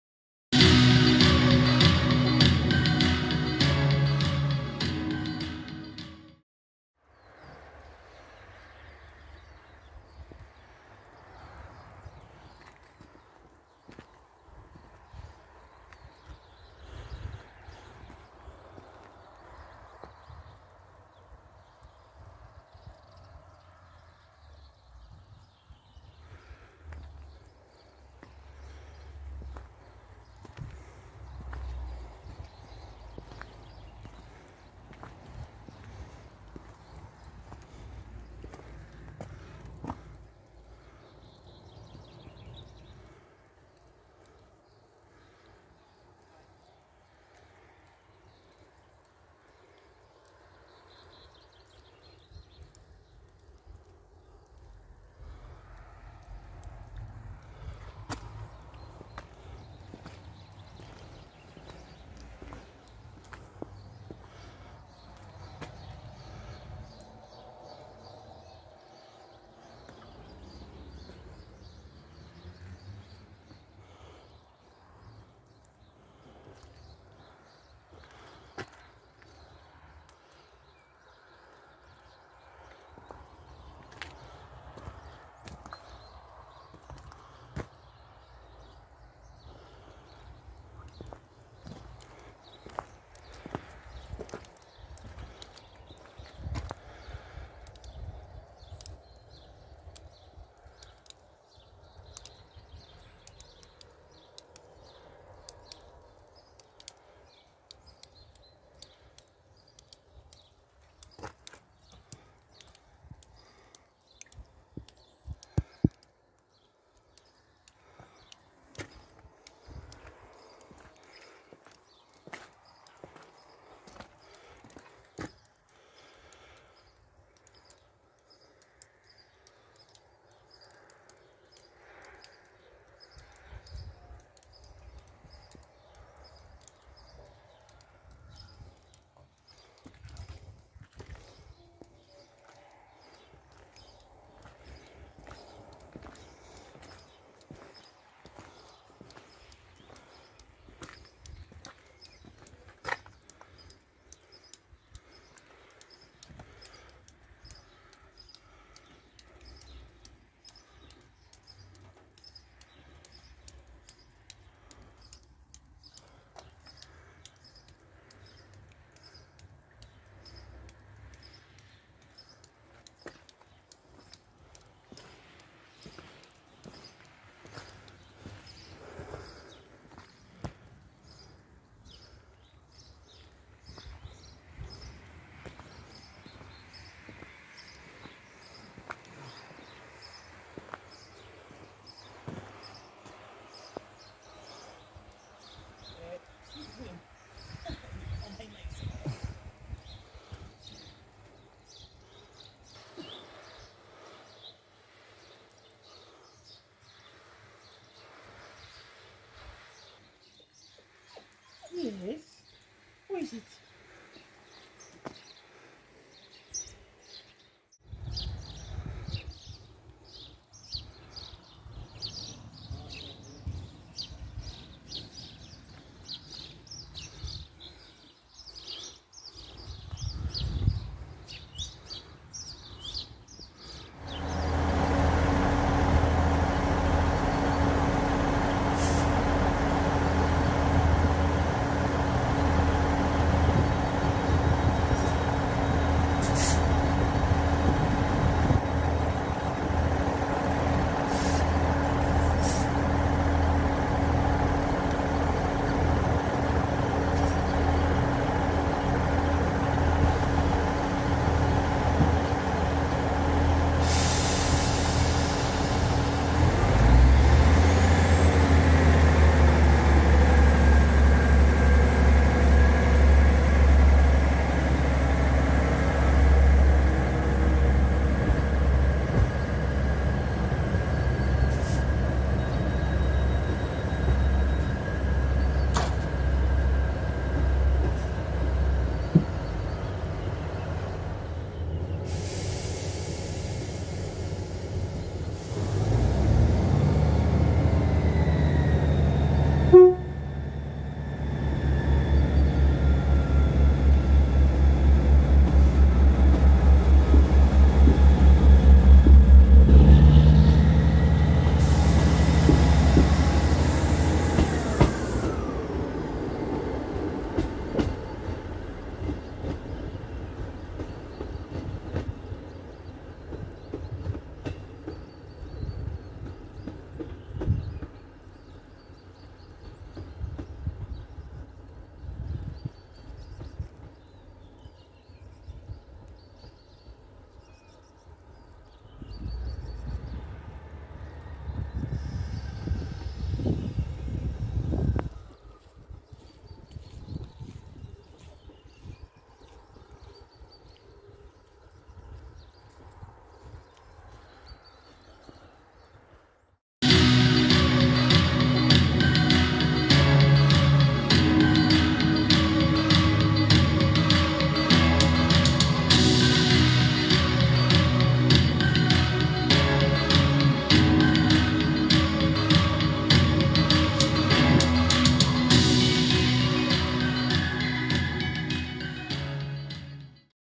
This is Achnasheen Station (Episode) 28
Achnasheen railway station is a remote railway station on the Kyle of Lochalsh Line. serving the village of Achnasheen  in the north of Scotland.

Scotrail Operate serives From Inverness to Kyle of Lochalsh. DMUs 158s.

This was recorded 2022

Caithness Rail Ramber.